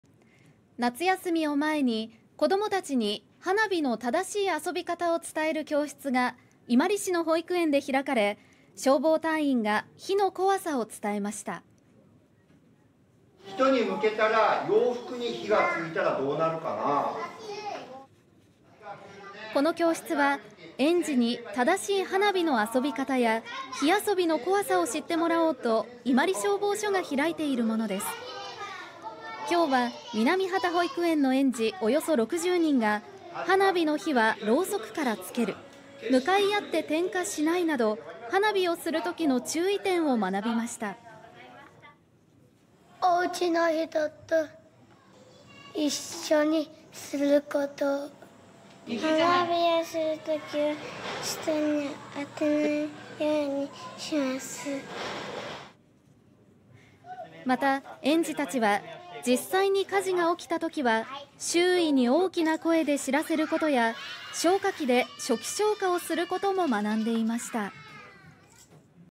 「人に向けてしない」消防隊員が園児に花火の正しい遊び方伝える"花火教室"【佐賀県伊万里市】 (23/07/07 17:30)
夏休みを前に子どもたちに花火の正しい遊び方を伝える教室が伊万里市の保育園で開かれ、消防隊員が火のこわさを伝えました。

【消防隊員】
「（花火を）人に向けて洋服に火が付いたらどうなるかな？」

この教室は、園児に正しい花火の遊び方や火遊びの怖さを知ってもらおうと、伊万里消防署が開いているものです。
7日は南波多保育園の園児約60人が「花火の火はろうそくからつける」「向かい合って点火しない」など花火をするときの注意点を学びました。

【園児】
「（花火は）おうちの人と一緒にする」
「花火をするときは人に当てないようにします」

また園児たちは、実際に火事が起きたときは周囲に大きな声で知らせることや、消火器で初期消火をすることも学んでいました。